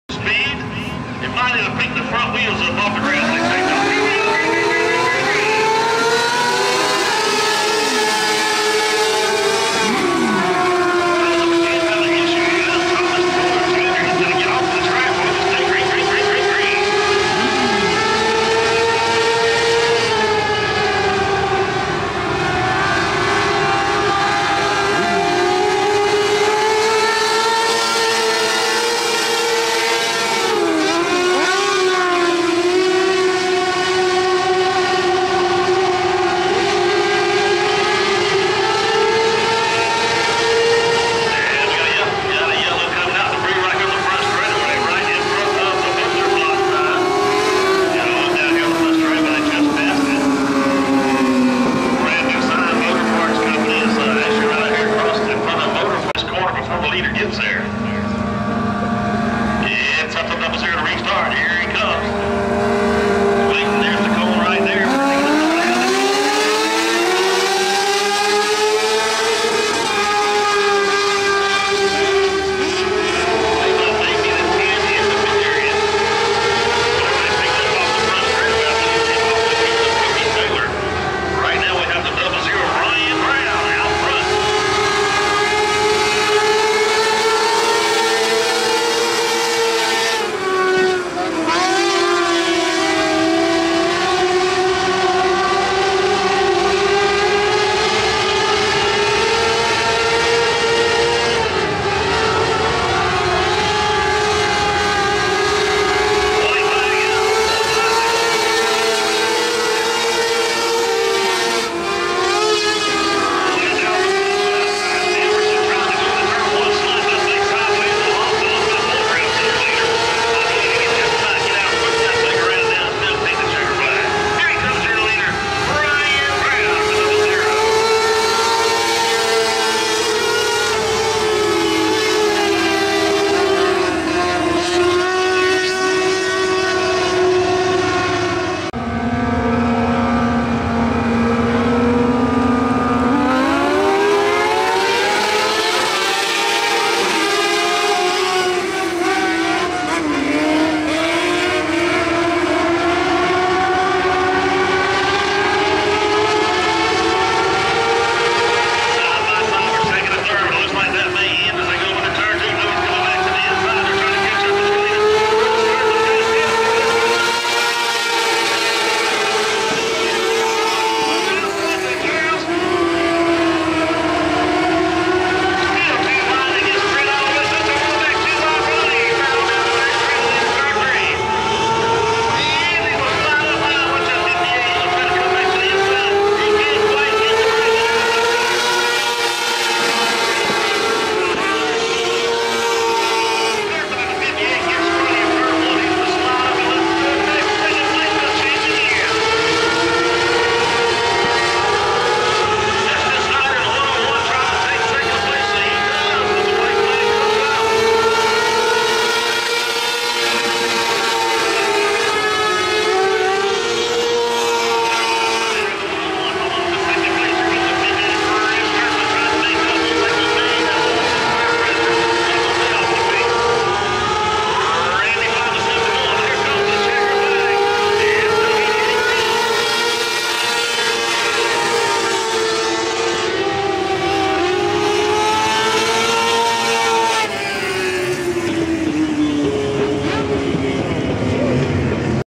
09/04/20 Mini Late Model Heat Race
Heat Race 1
Row 1 10 Cody Taylor                00 Ryan Brown
Row 2  01 Thomas Cooler Jr    22 Brad Amerson
Heat Race 2
Row 1  58  Chris Gerdmann         101 Dustin Snider
Row 2  18  Tyler Keel                     71  Randy Fly